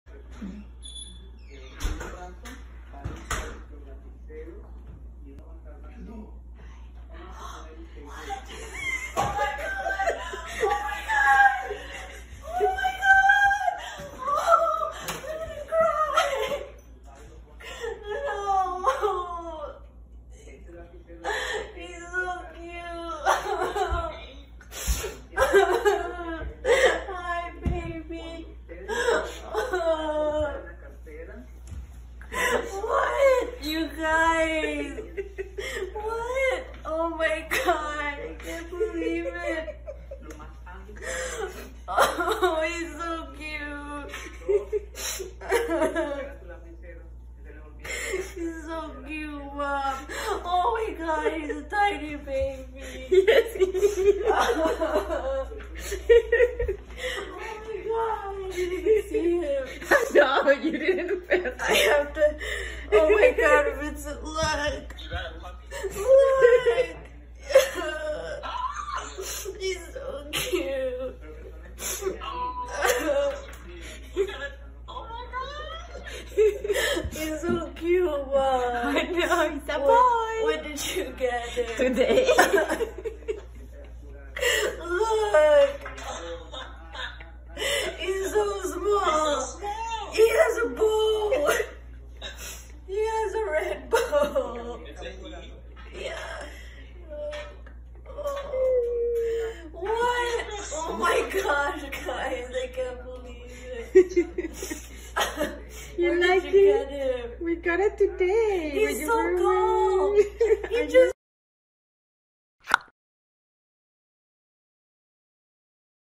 Emotional Puppy Surprise - Try Not To Cry || WooGlobe
I came home and my parents surprised me with a puppy. This puppy surprise is a bichon Frise named forest. I was on the phone with my boyfriend when I walked inside so after freaking out I began to FaceTime him to show him the puppy. Sadly Forest now has parvovirus and is fighting for his life. I’m fundraising for his costly vet bills by offering to draw pictures of people’s pets since I am an illustrator. I love this puppy so much already.

Copyright - WooGlobe.